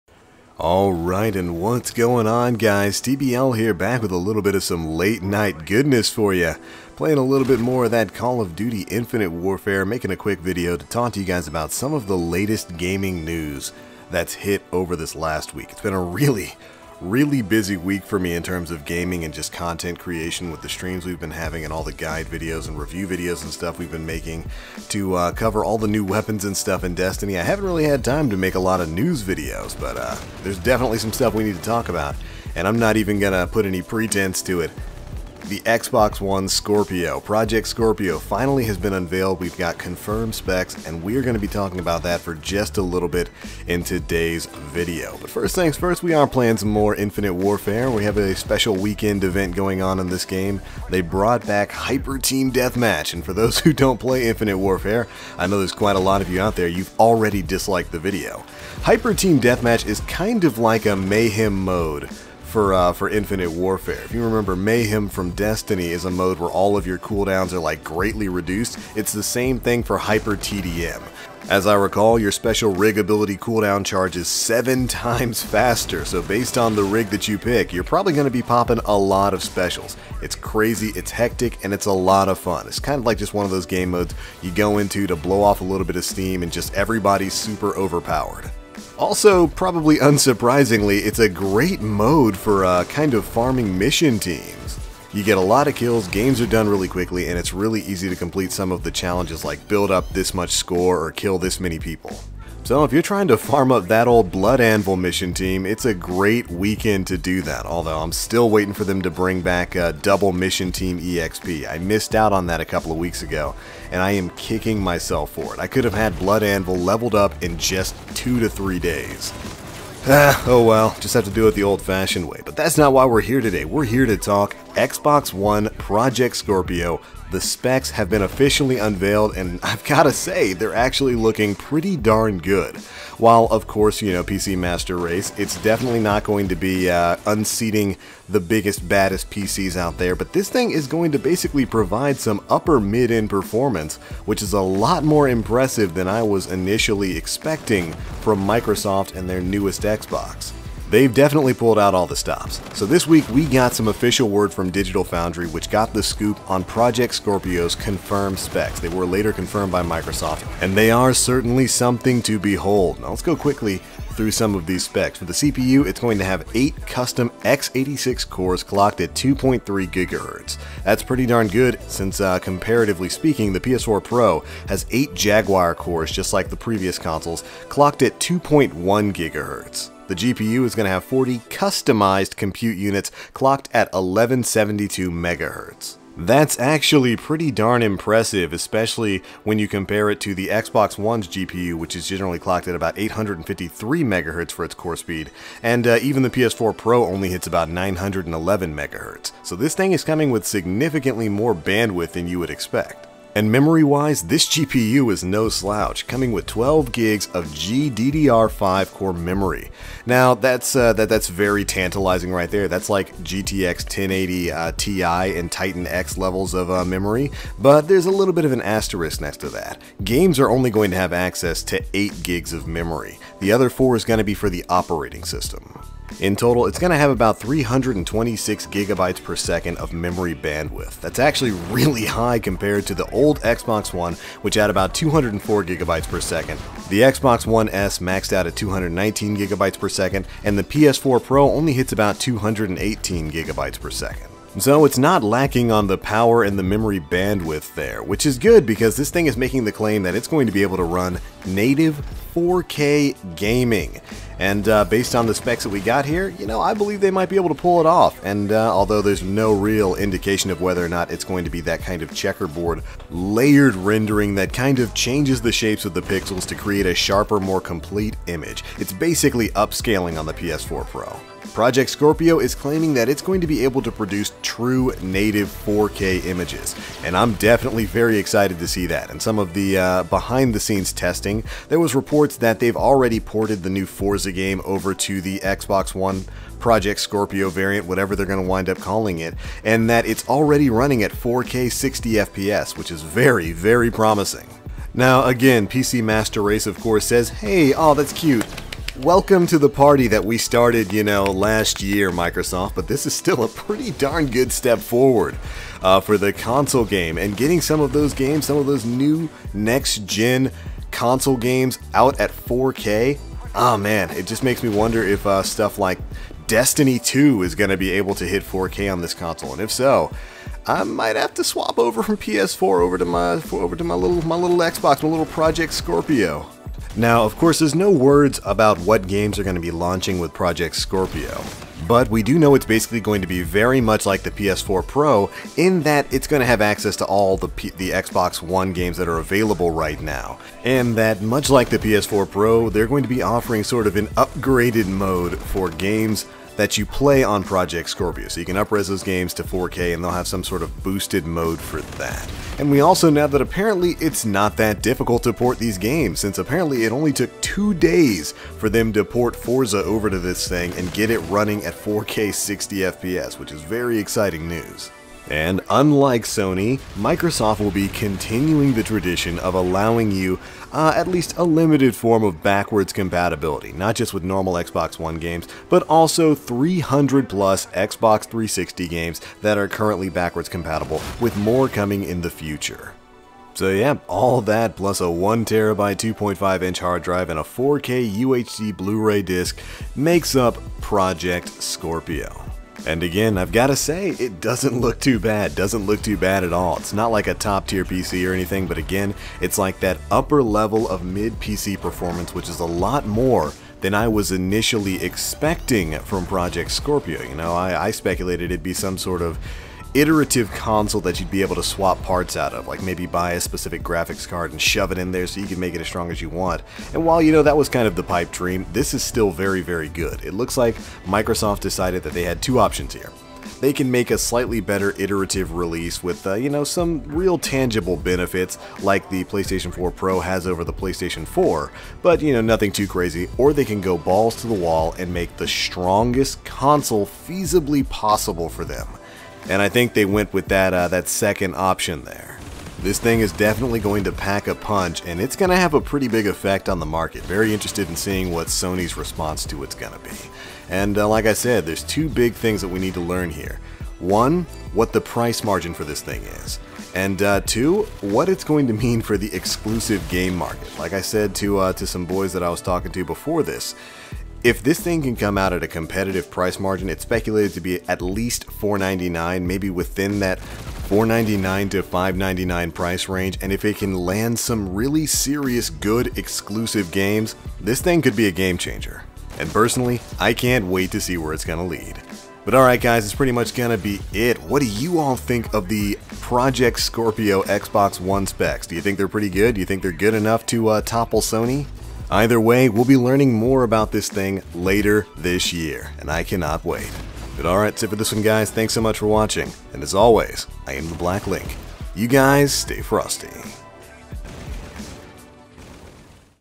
Project Scorpio Specs Revealed? Native 4K Console Gaming Incoming?
So the final specs for Microsoft's Project Scorpio Xbox One iterative release have finally been unveiled, and they're looking pretty beefy!